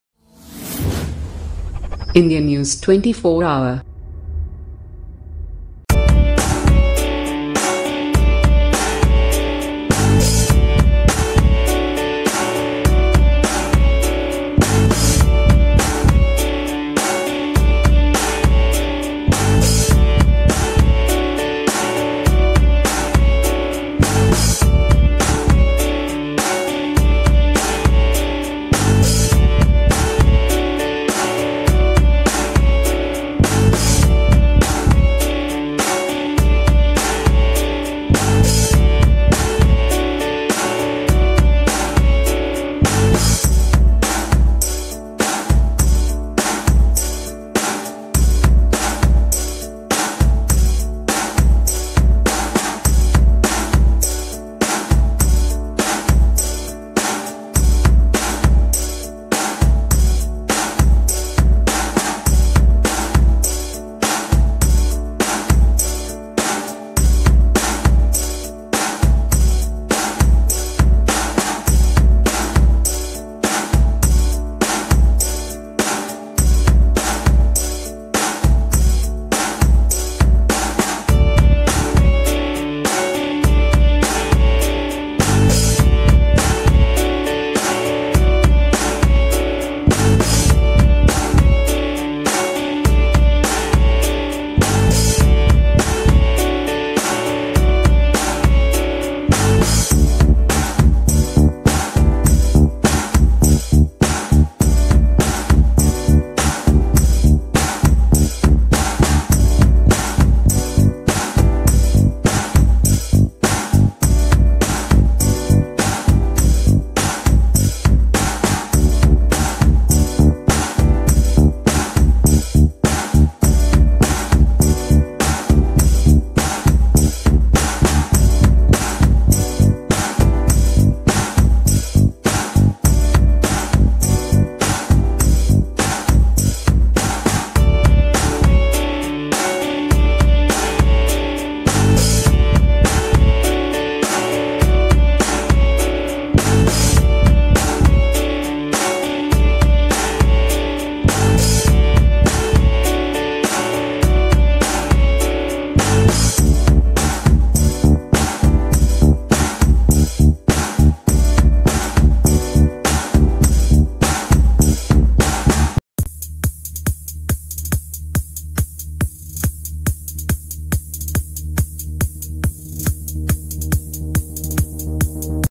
After shock rk nagar poll defeat, aiadmk cracks whip against nine dhinakaran men
Stung by the RK Nagar bypoll defeat, the ruling AIADMK on Monday cracked the whip against nine party functionaries, sacking four supporters of rebel leader TTV Dhinakaran from their posts and ousting five others from the party. PTI | Dec 25, 2017, 16...